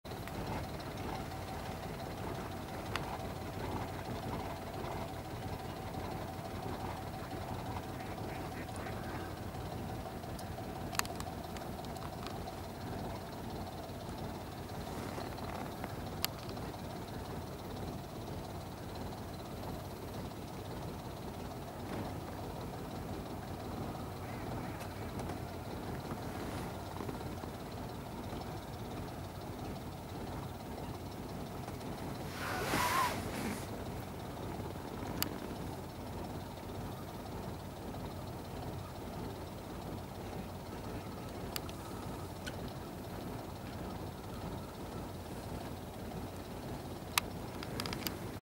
Zwarte Stern, Weerribben
Chlidonias niger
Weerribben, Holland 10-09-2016
Super vaartocht met Kuipers en de dames in de Weerriben!!
Black Tern in Holland